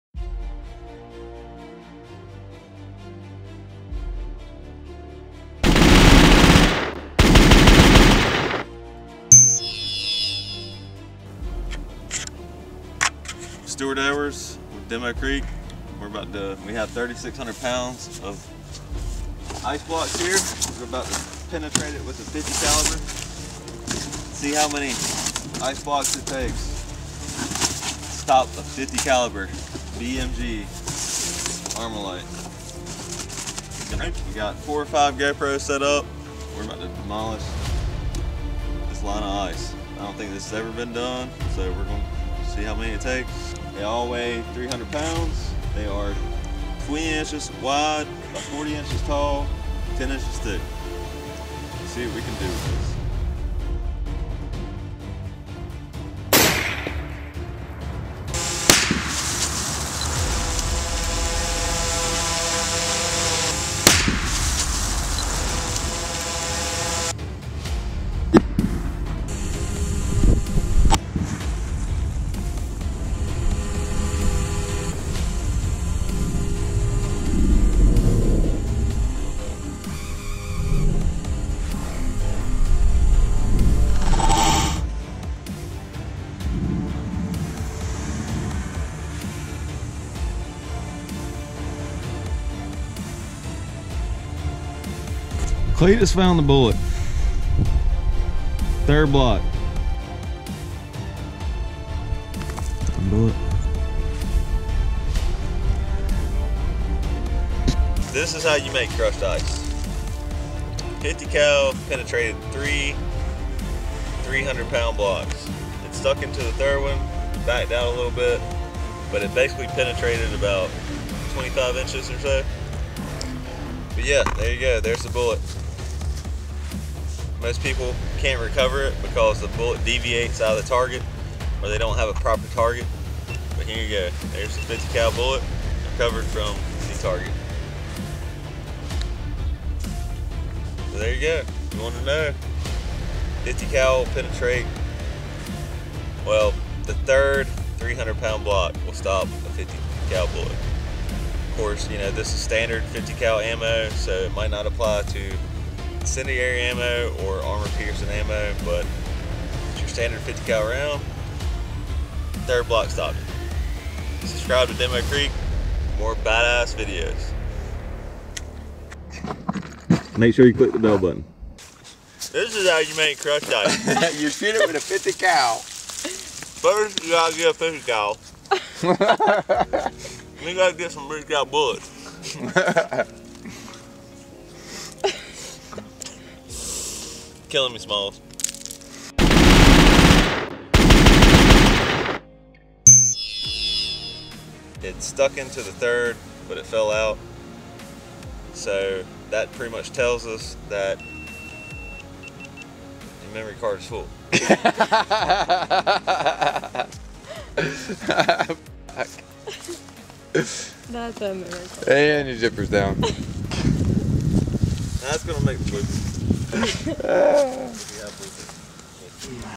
50 BMG VS 3600lbs of ICE BLOCKS 10FT LONG
50 BMG VS 3600lbs of ICE BLOCKS 10FT LONG